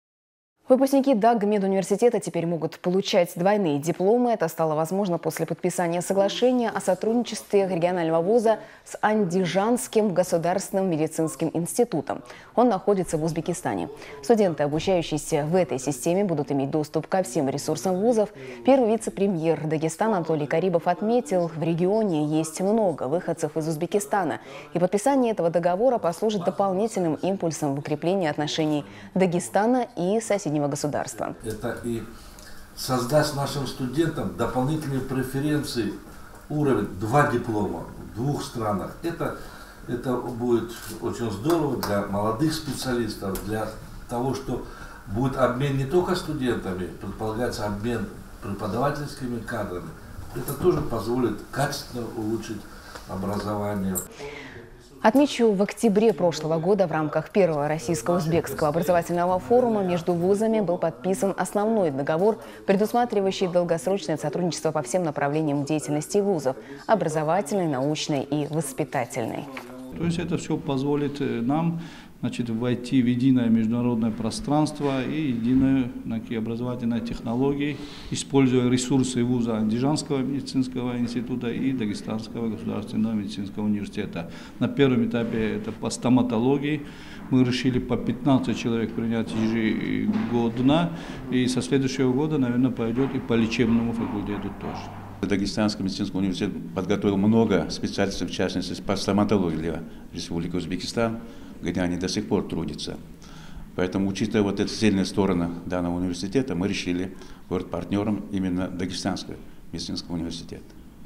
Выпускники ДГМУ теперь могут получать двойные дипломы
Выпускники Дагмедуниверситета теперь могут получать двойные дипломы. Это стало возможным после подписания соглашения о сотрудничестве регионального вуза с Андижанским государственным медицинским институтом. Он находится в Узбекистане.